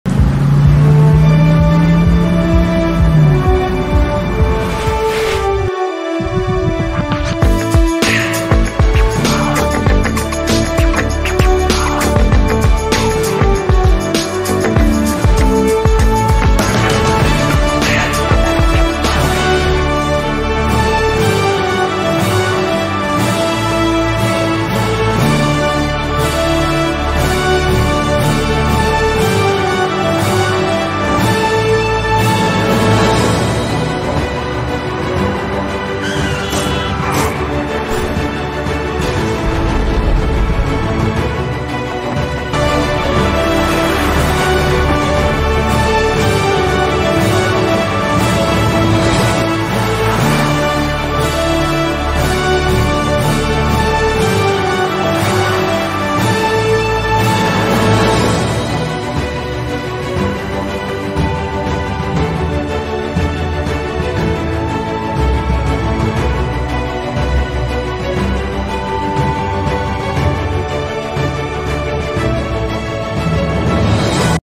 Nominasi Animasi 3D Kelas XI MM Semester 3 2022-2023
Nominasi ini diberikan untuk siswa/i yang memiliki kompetensi keahlian sesuai bakat nya dan kesungguhan dalam belajar.